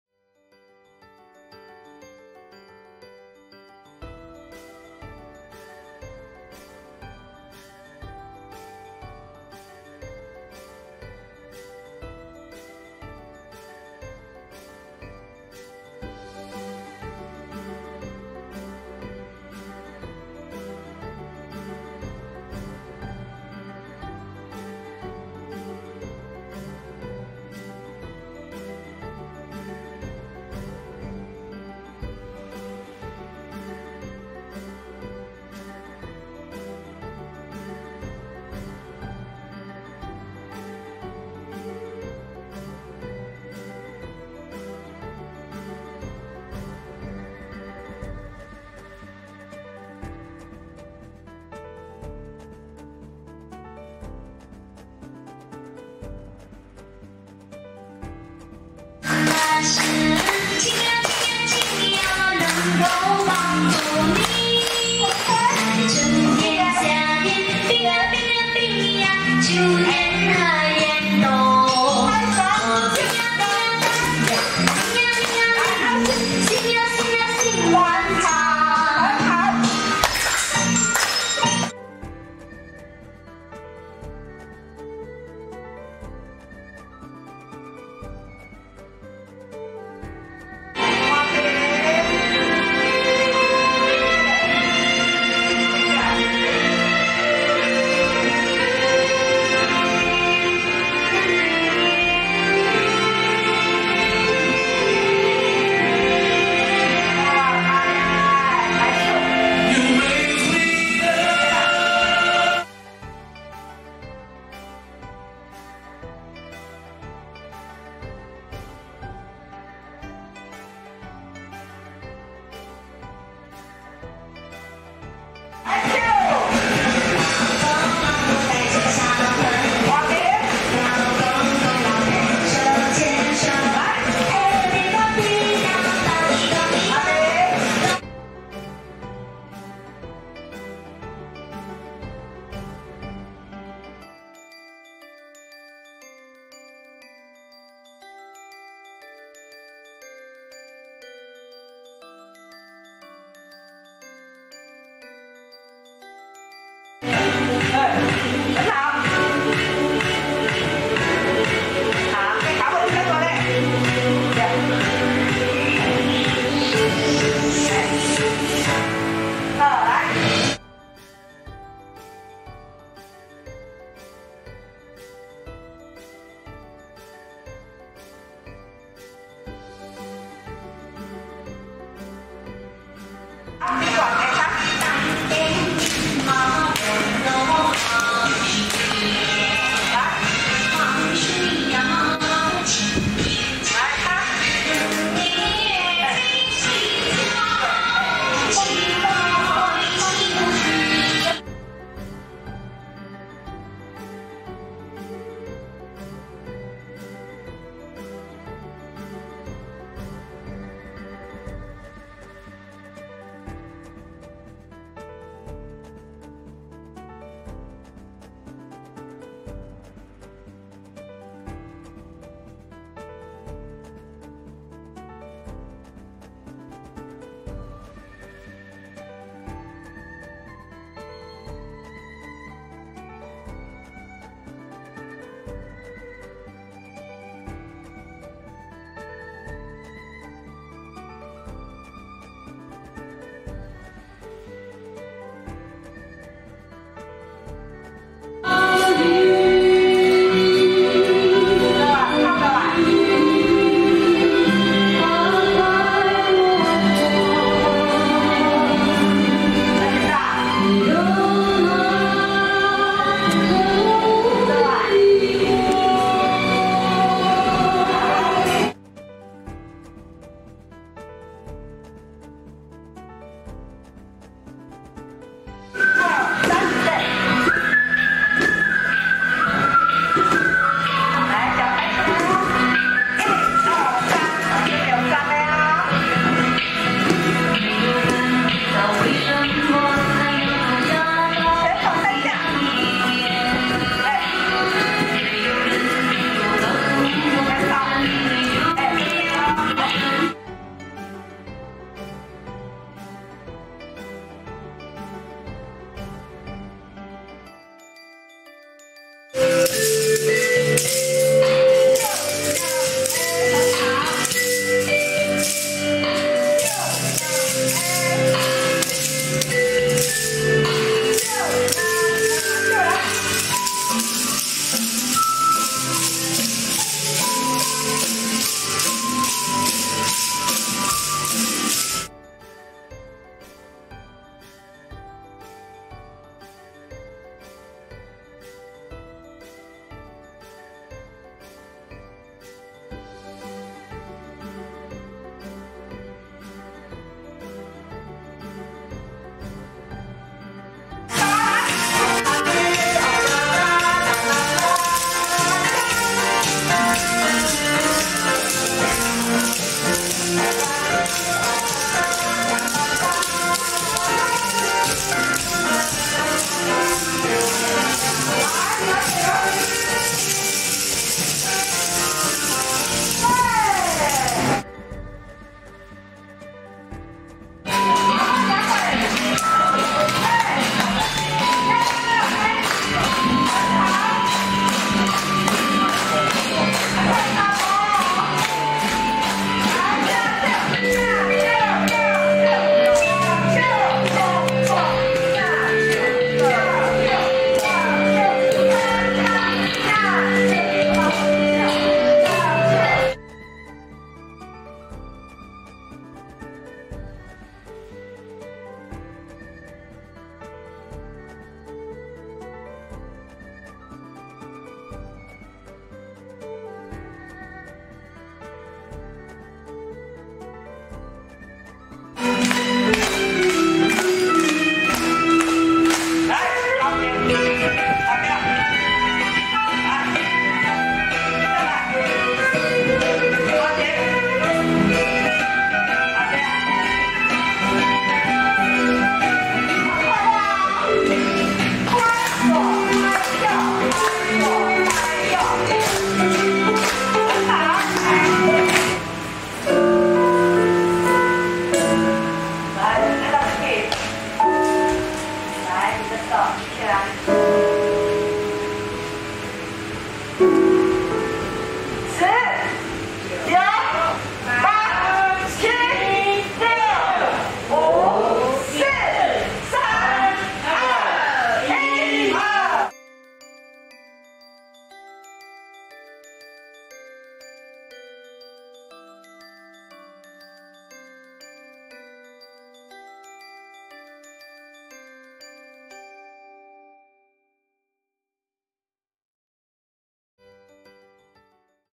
烏竹社區巷弄長照站 113.08.05 辦理預防及延緩失能課程（2）能量律動健康操 05/12
烏竹社區巷弄長照站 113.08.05 辦理預防及延緩失能課程（2）能量律動健康操 05/12、阻力運動-水啞鈴、據點共餐（感謝金牌便當贊助共餐）。